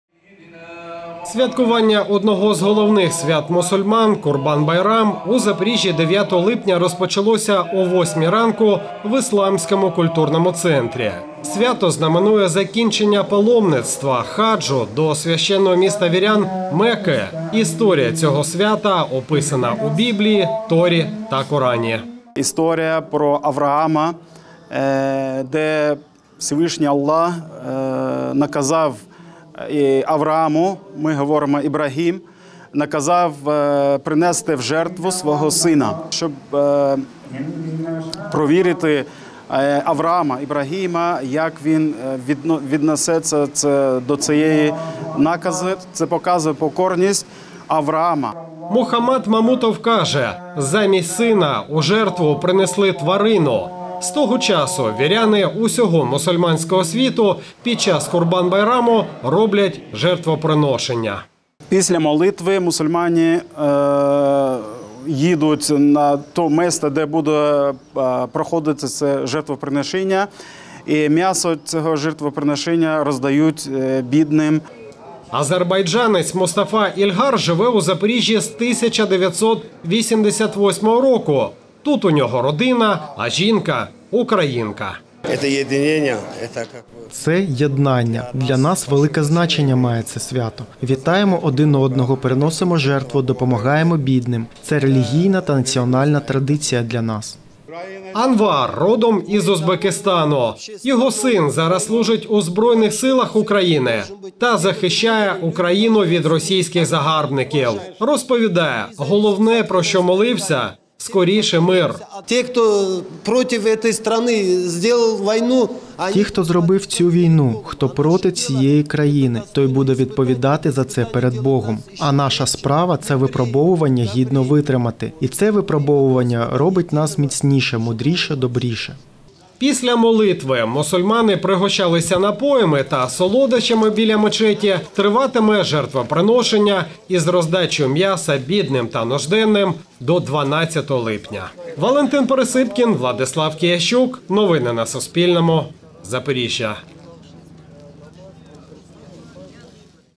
Як у Запоріжжі відзначають Курбан-байрам | Новини
У запорізькому Ісламському культурному центрі 9 липня відзначили Курбан-Байрам. Одне з головних свят для мусульман прийшло відзначити близько півсотні людей. У мечеті здійснили намаз – молитву, слухали святкову проповідь, потім – спілкувалися й вітали один одного. Журналісти Суспільного бачили як це відбувалось.